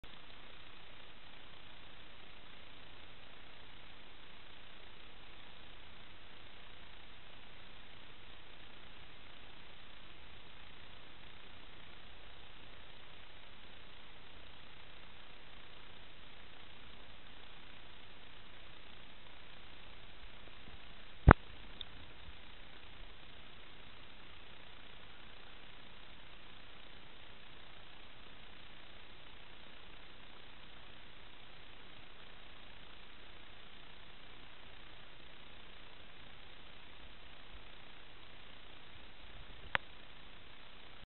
Tucan fly
Una de las mejores moscas para pescar cabeza amarga,tucan fly de goma eva en anzuelos nº 6 y 8